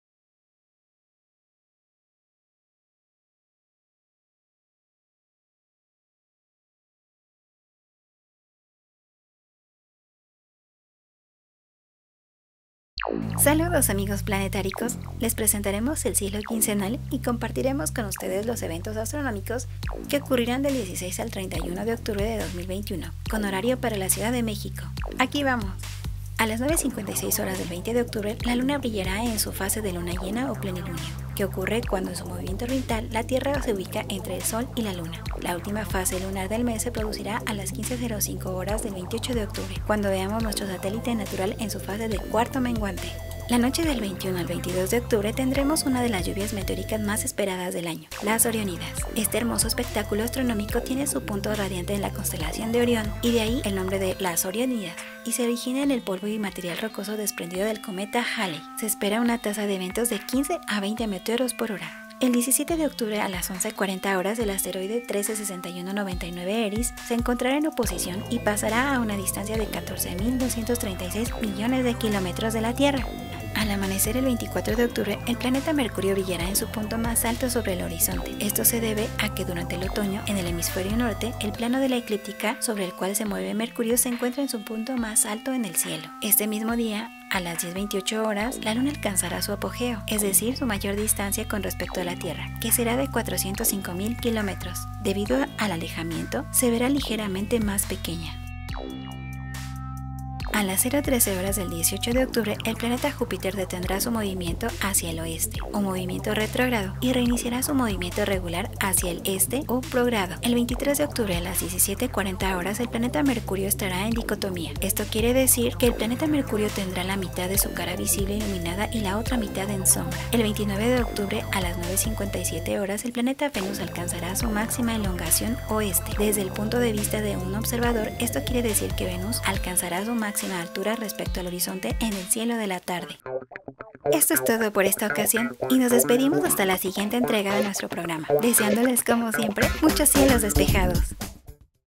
🌕Eventos astronómicos de la segunda quincena de octubre🪐
¡Vive con nosotros la astronomía y la ciencia!
📌Conoce los eventos astronómicos que se darán del 1 al 15 de Octubre 2021.